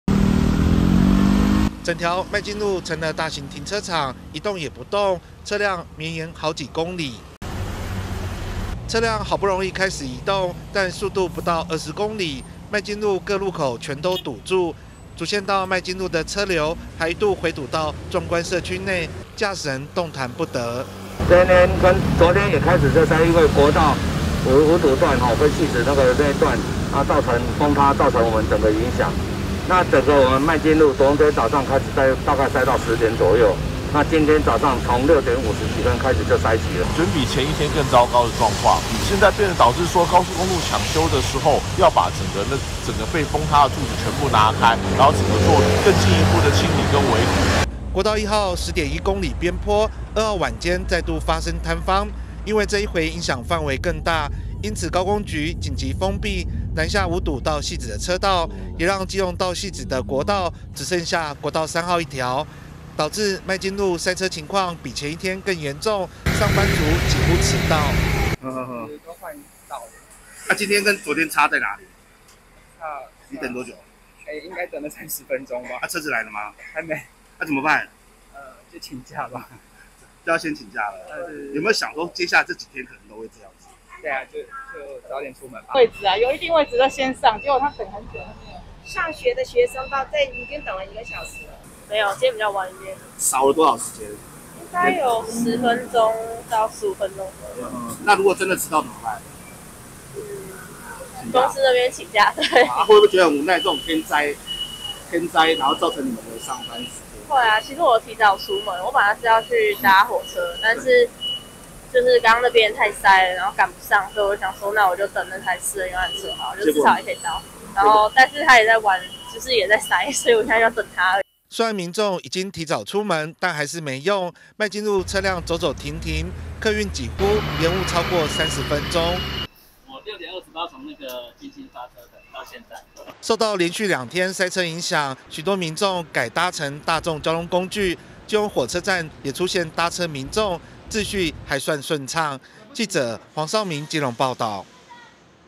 國1走山｜基隆通勤族崩潰！等客運+塞車=2hrs 遲到了
(中嘉新聞記者黃紹銘/基隆採訪報導) 國1南向汐止匝道邊坡，第2度走山，國1五堵到汐止主線封閉，不僅開車族崩潰，通勤族也是叫苦連天，提早出門也沒用，光等車就要1小時，甚至有民眾等不到客運，改前往火車站搭台鐵上班課，幾乎都遲到。